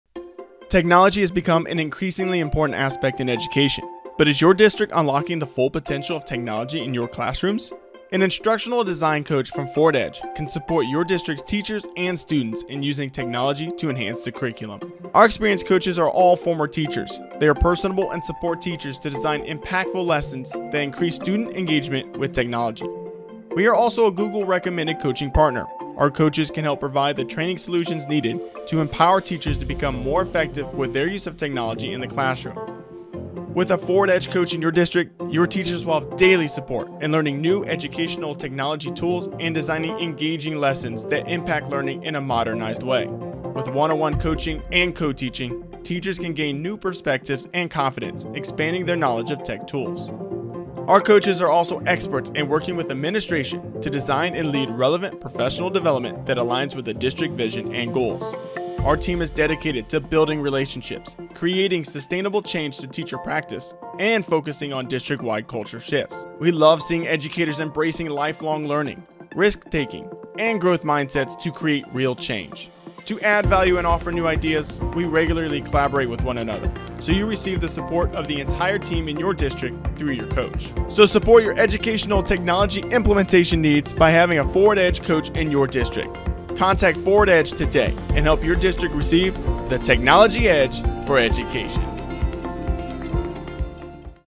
Curriculum & Integration Department | Instructional Design Coaches
Forward Edge's team of Instructional Design Coaches or Technology Coaches help teachers improve their use of technology by introducing new strategies on implementation, exploring different classroom approaches and finding resources to support the day-to-day activities of various teachers. Watch the video to learn more.